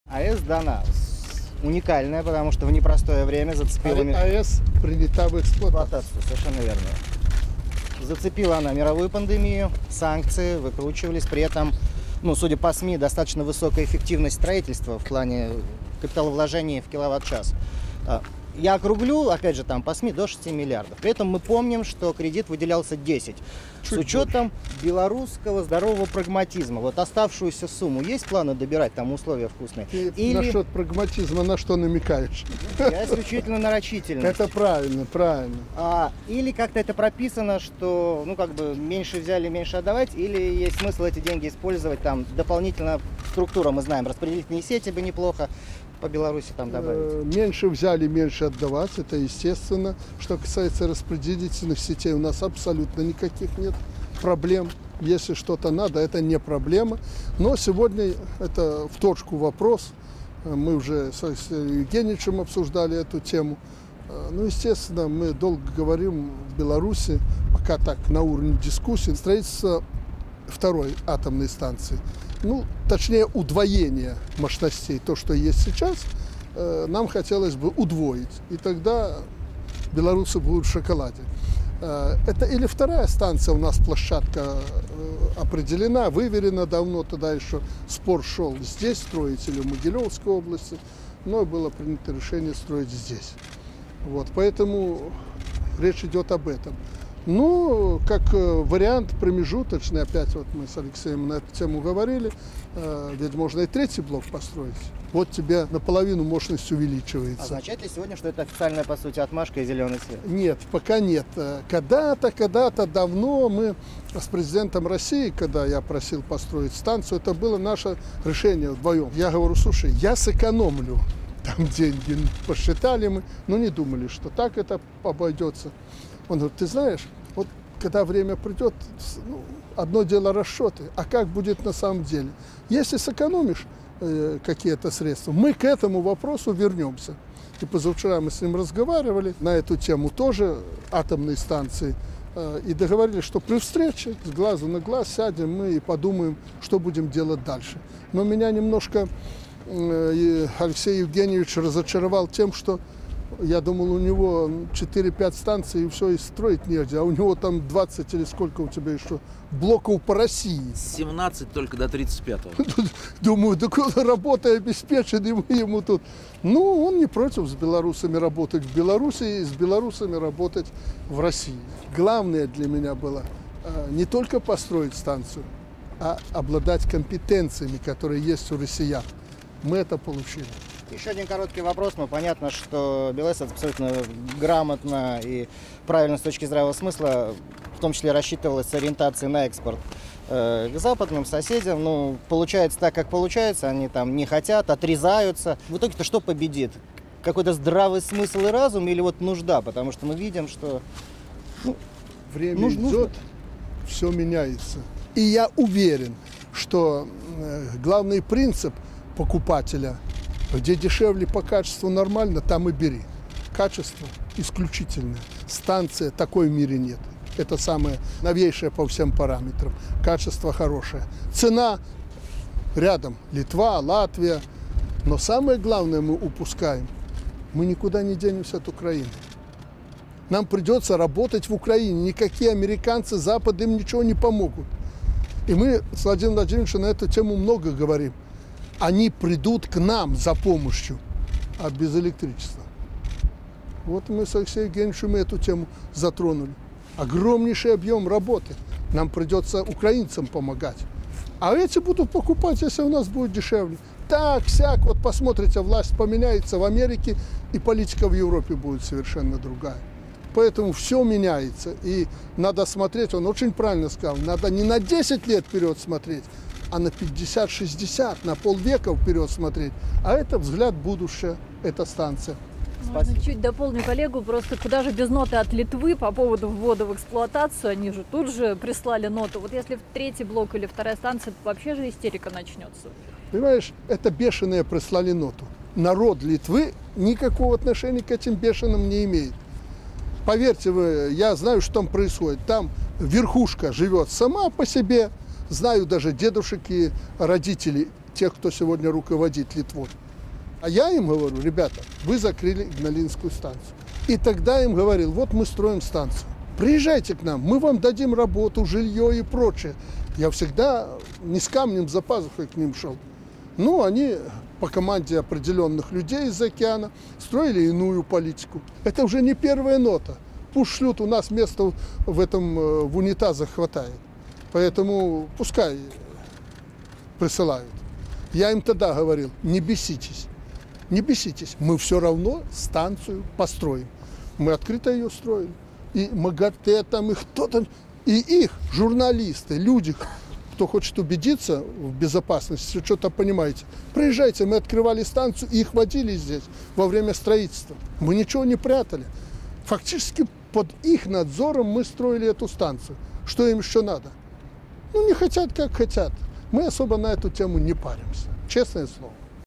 Лукашенко: Пусть шлют! У нас места в унитазах хватает! Поэтому пускай присылают!
Лукашенко: Это бешеные прислали ноту. Народ Литвы никакого отношения к этим бешеным не имеет. Поверьте, я знаю, что там происходит. Там верхушка живет сама по себе! Знаю даже дедушек и родителей тех, кто сегодня руководит Литвой!

Во время посещения БелАЭС 3 ноября Президент ответил на вопросы российских журналистов.